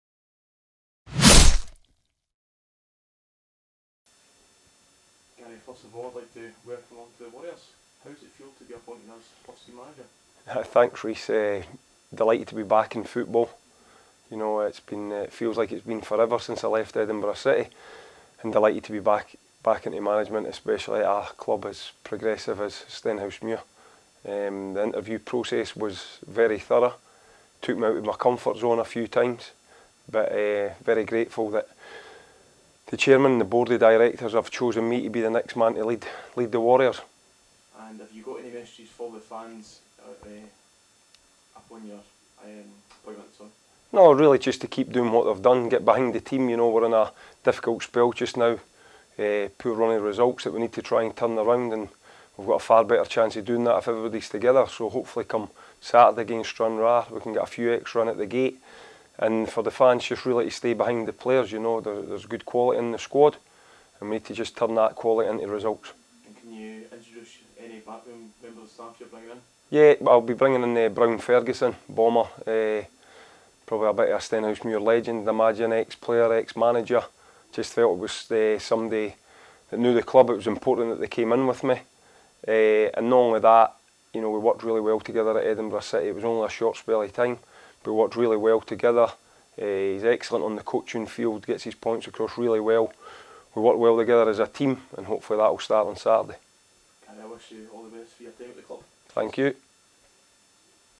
WarriorsTV - Manager's Interview - Gary Naysmith
WarriorsTV spoke with new Stenhousemuir FC Manager Gary Naysmith following his recent appointment.